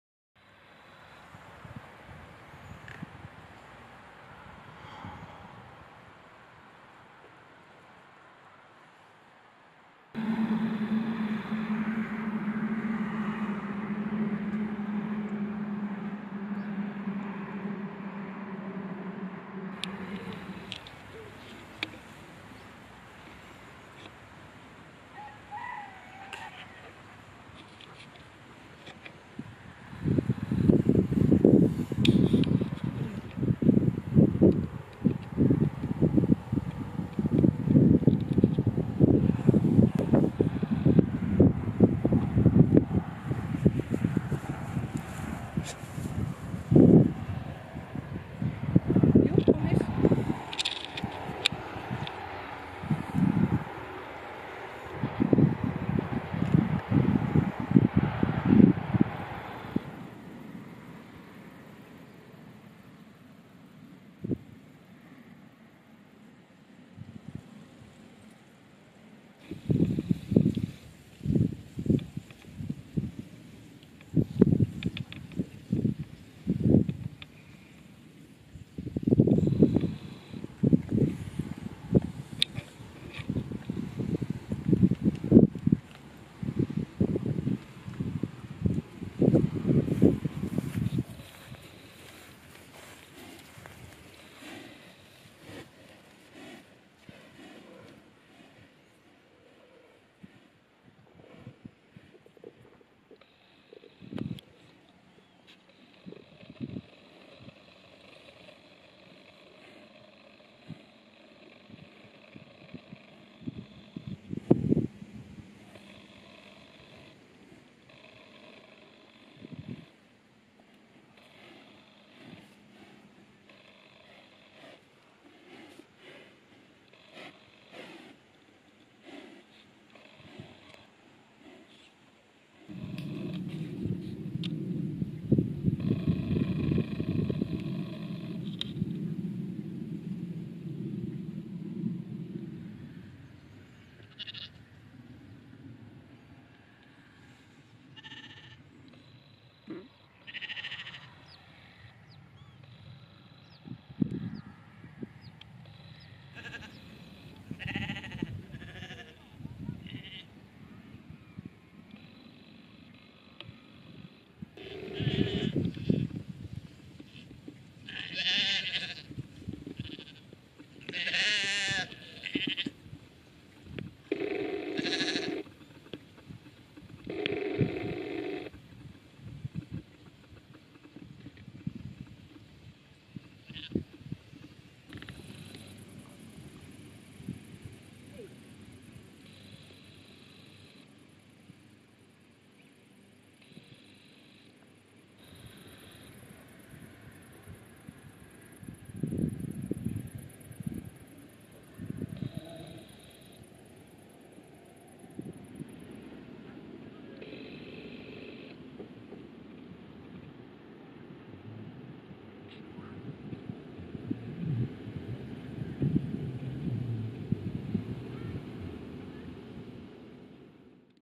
FHD 1080p 50fps demo Panasonic Lumix DMC GX800
No audio or video editing , just video as it came from the camera